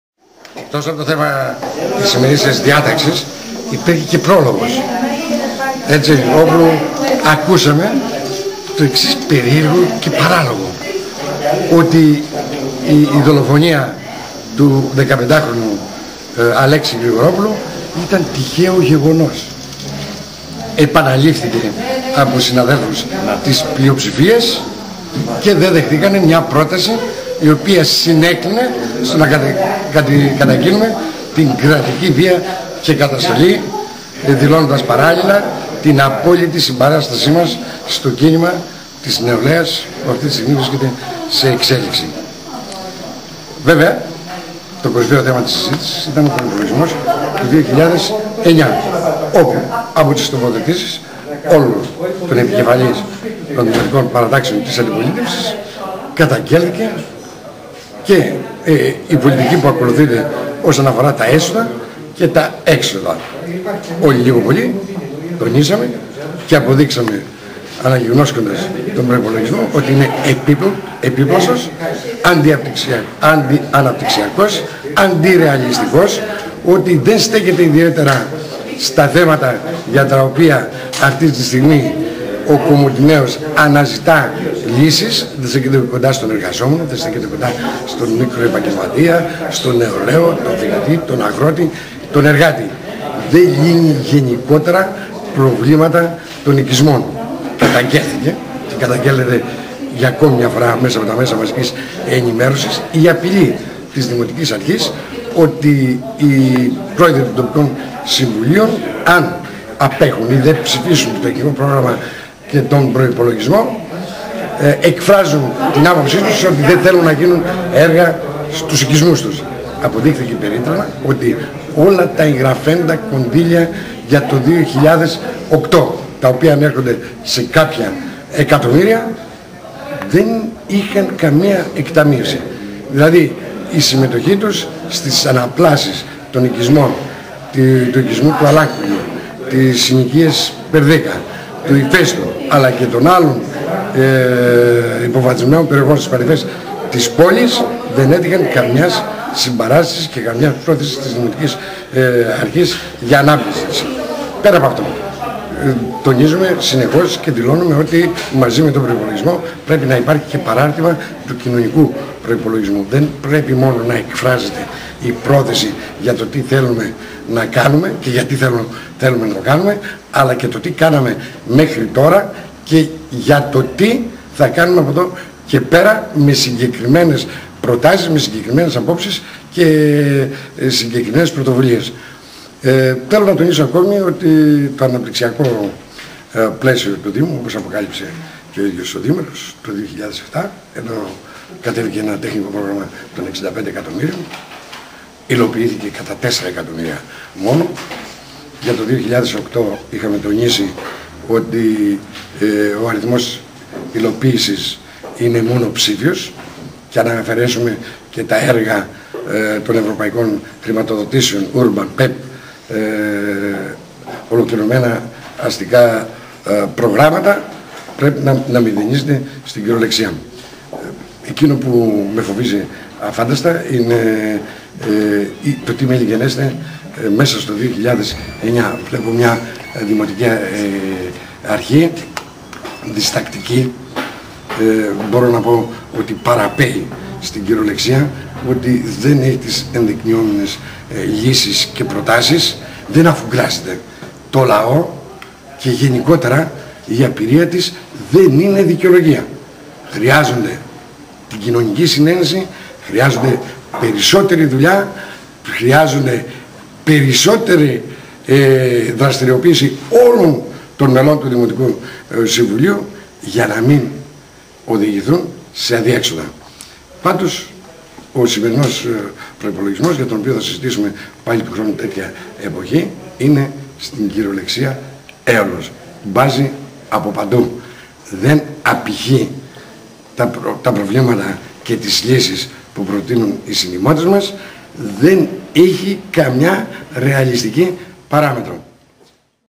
Βαβατσικλής Τάσος (16 Δεκεμβρίου 2008)
Εκτός από το θέμα που θα συζητούσαμε σήμερα και αφορούσε τον Πορϋπολογισμού του 2009, υπήρχε και πρόλογος. Ακούσαμε το εξής περίεργο και παράλογο. Ότι η δολοφονία του 15χρονου μαθητή Αλέξη Γρηγορόπουλου ήταν «τυχαίο γεγονός»! Επαναλήφθηκε από συναδέλφους της πλειοψηφίας και δεν δεχθήκανε μια πρόταση, η οποία συνέκλινε στο να καταγγείλουμε την κρατική βία και καταστολή, δηλώνοντας παράλληλα την απόλυτη συμπαράσταση μας στο κίνημα της νεολαίας που αυτή την στιγμή βρίσκεται σε εξέλιξη.
Το κορυφαίο θέμα της συζήτησης ήταν ο προϋπολογισμός του 2009, όπου από τις τοποθετήσεις όλων των επικεφαλής των δημοτικών παρατάξεων της αντιπολίτευσης καταγγέλθηκε η πολιτική, όσον αφορά τα έσοδα και τα έξοδα. Όλοι τονίσαμε και αποδείξαμε αναγιγνώσκοντας τον προϋπολογισμό ότι είναι επίπλαστος, αντιαναπτυξιακός, αντιρεαλιστικός, δεν στέκεται ιδιαίτερα στα θέματα για τα οποία αυτήν την στιγμή ο δημότης αναζητά λύσεις. Δεν στέκεται κοντά στον εργαζόμενο, δεν στέκεται κοντά στον επαγγελματία, στον νεολαίο, τον φοιτητή, τον αγρότη, τον εργάτη. Δεν λύνει γενικότερα προβλήματα των οικισμών. Καταγγέλθηκε και καταγγέλλετε για μια ακόμη φορά μέσα από τα ΜΜΕ, η απειλή της δημοτικής αρχής, ότι οι πρόεδροι των τοπικών συμβουλίων αν απέχουν ή δεν ψηφίσουν το τεχνικό πρόγραμμα και τον προϋπολογισμό, εκφράζουν την άποψη τους ότι δεν θέλουν να γίνει έργα στους οικισμούς τους. Δηλαδή η συμμετοχή τους στις αναπλάσεις των οικισμών Αλάν Κουγιου, Περδίκα, Ηφαίστου, καθώς και των άλλων υποβαθμισμένων οικισμών του Δήμου Κομοτηνής, δεν έτυχαν καμιάς συμπαράστασης και καμιάς πρόθεσης της δημοτικής αρχής για ανάπτυξη.